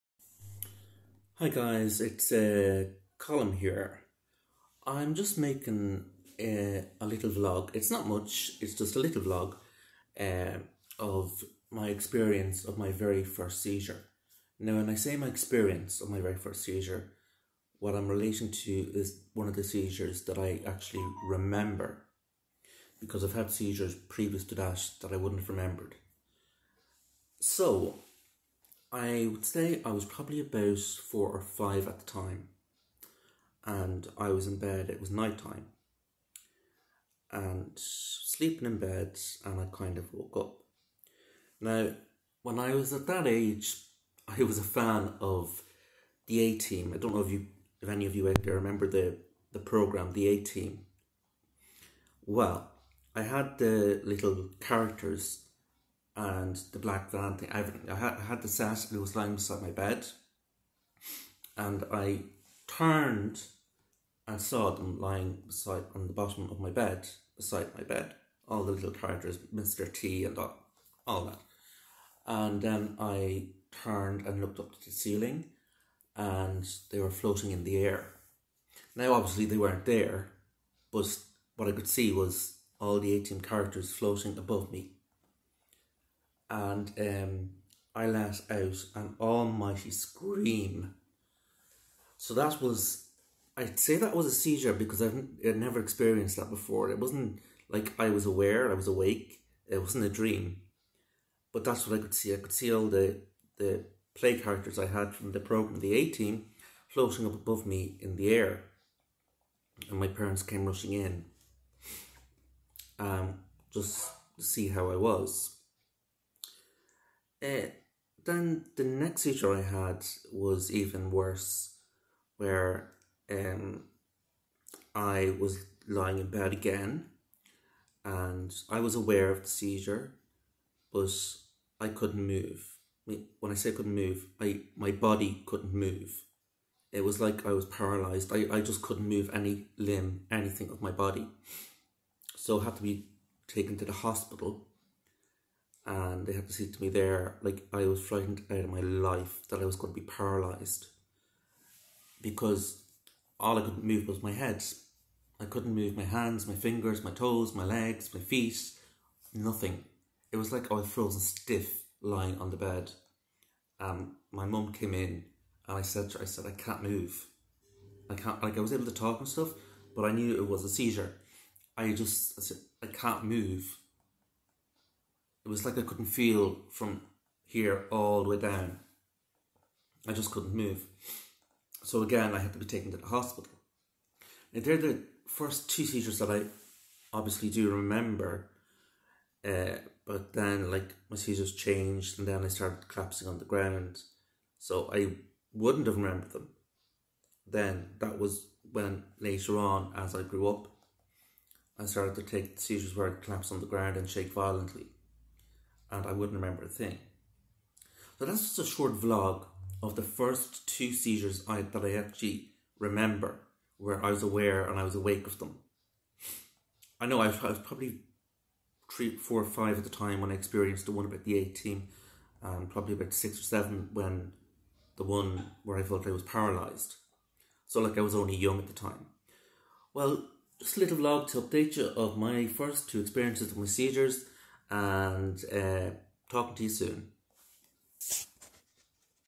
Remembering my very first seziure
Just a small vlog of my experience when i had my very first seziure